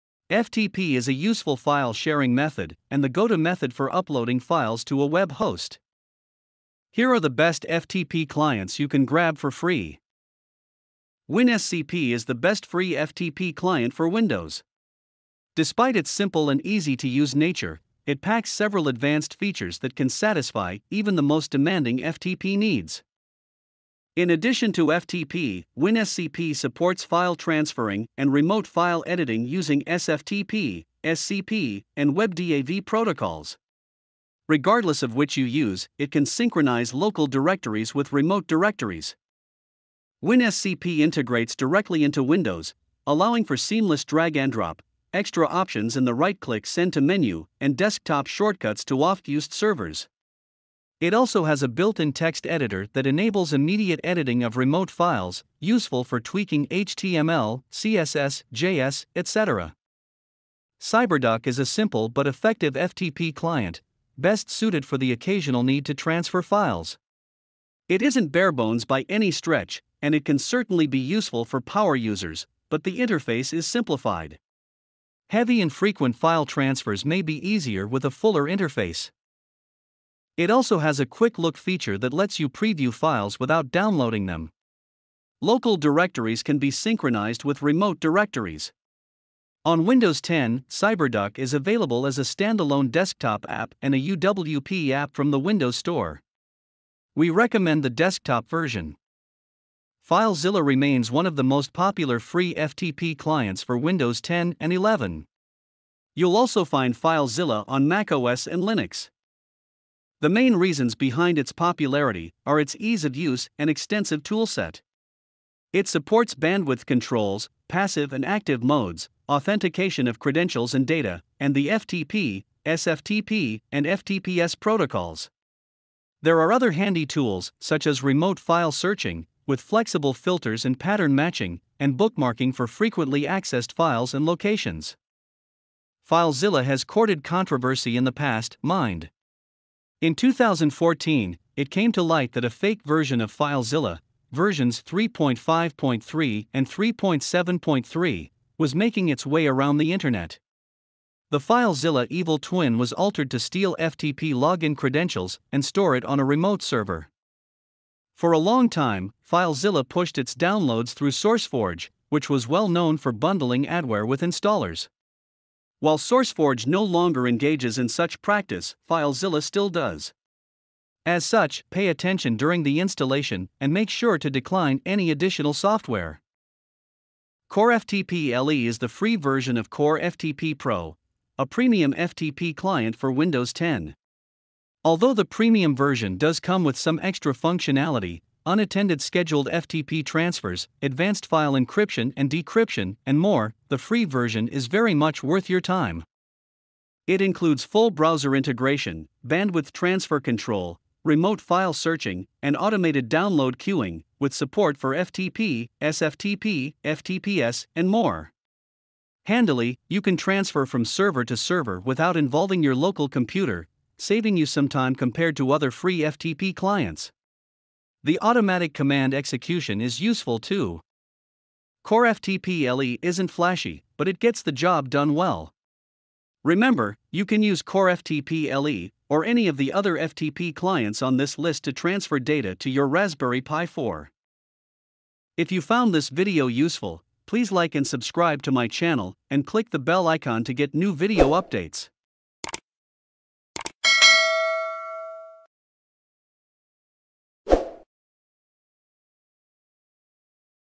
The Best Free FTP Clients for Windows
FTP is a useful file sharing method and the go-to method for uploading files to a web host.
Here are the best free FTP clients for Windows.